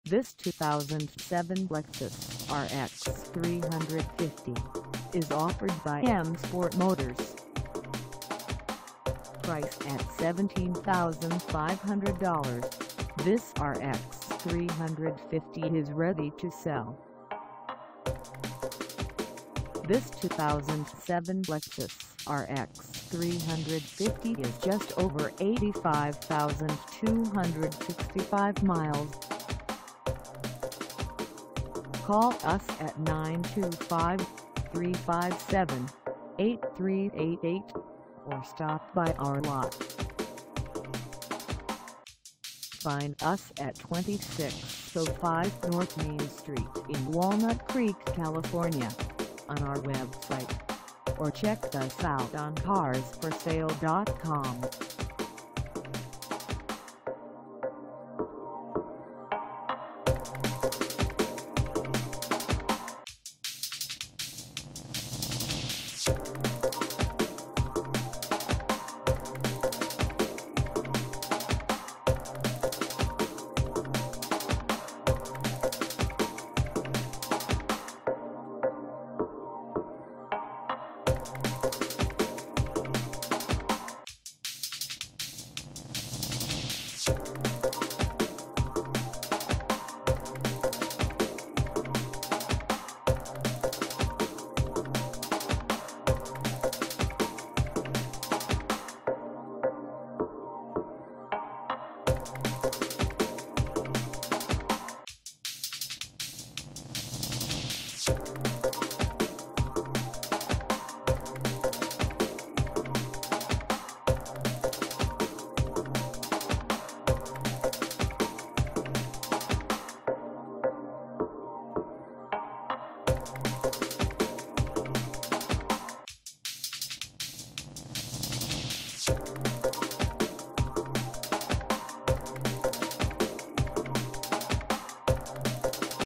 2007 Lexus RX 350 for sale in Walnut Creek, CA 94597 at the
This 2007 Lexus RX 350 is for sale in Walnut Creek, CA 94597 at M Sport Motors.
Engine: V6 3.5L V6
Transmission: Automatic 5-Speed
Color: Blue
Mileage: 85265
Contact M Sport Motors 2605 North Main St. Walnut Creek, CA 94597 to test drive this 2007 Lexus RX 350  today.